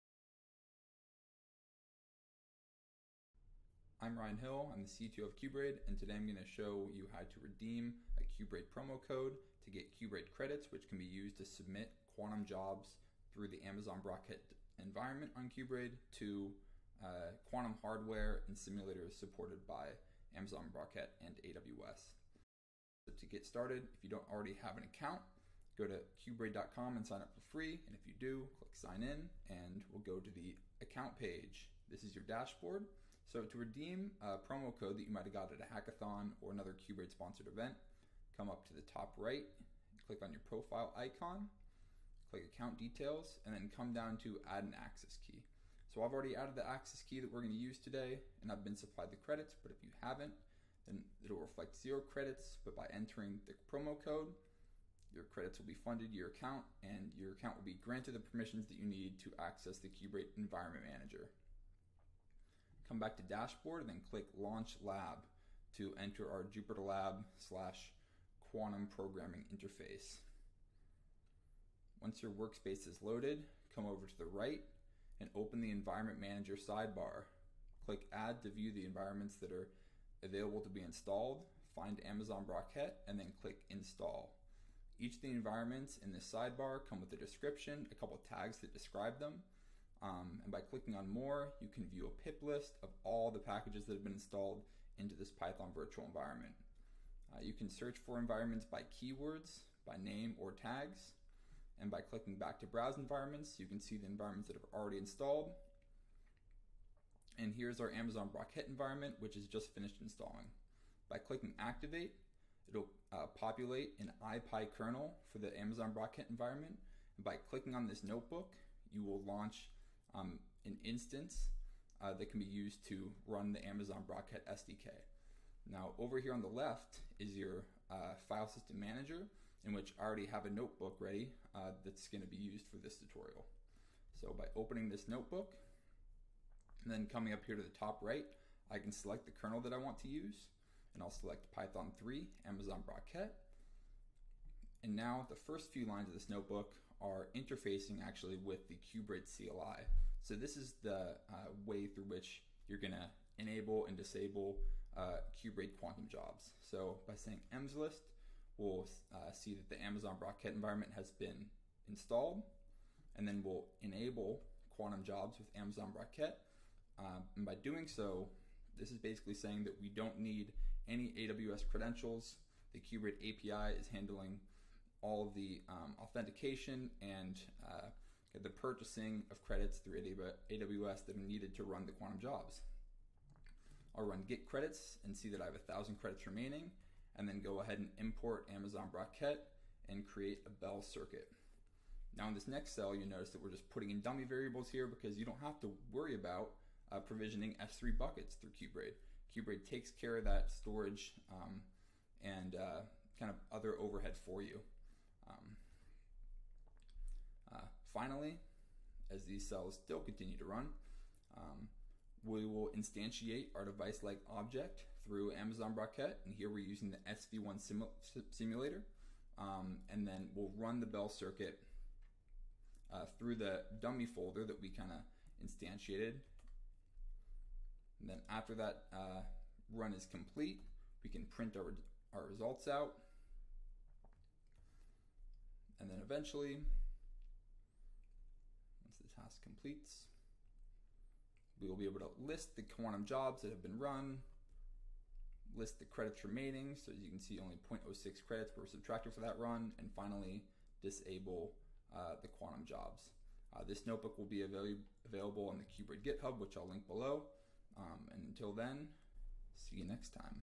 Using access codes to connect to quantum devices on qBraid | Demo
qBraid is a cloud-based platform for quantum computing. It provides software tools for researchers and developers in quantum, as well as access to quantum hardware.

This demo outlines how to redeem credits with an access code on qBraid, and how to use those credits when submitting quantum jobs to simulators and QPUs through Amazon Braket. The notebook / code used in this demo is available

Amazon Braket Supported Devices

0:00 Intro / login to qBraid account page
0:32 Redeem access / promo code
1:11 Install Amazon Braket environment
2:13 Select kernel and enable qBraid quantum jobs
3:03 Construct circuit and submit quantum job